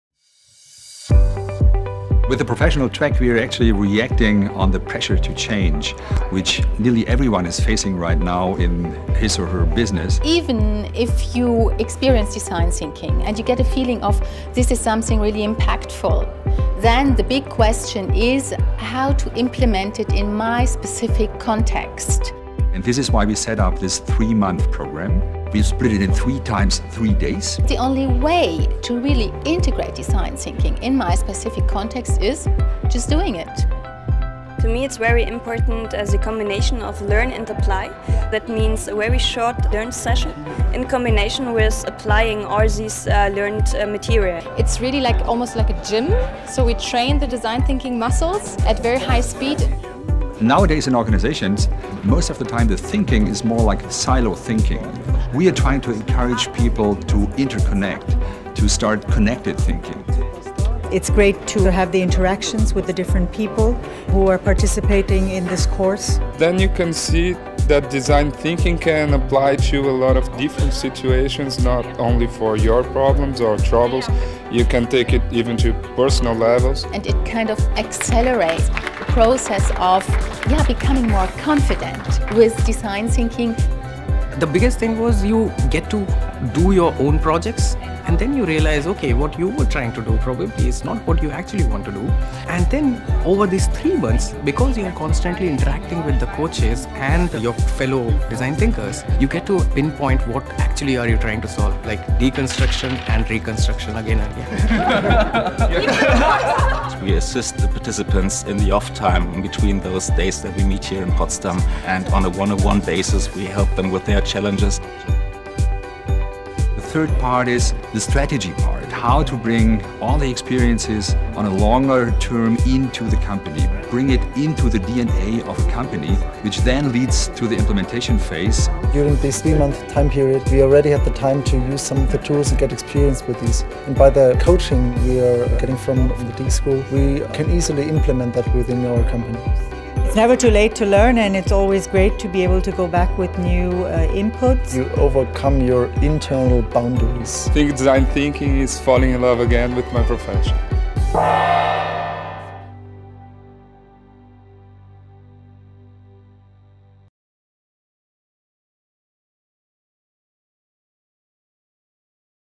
Design Thinking for Professionals: How Organizations Can Learn to Manage Complexity
The Design Thinking Professional Track was co-created by the HPI School of Design Thinking with Blue Chip companies. It offers an intensive and practical training for innovators and decision-makers in all kinds of organizations. The course enables participants to develop creative solutions to the complex challenges in their various work environments.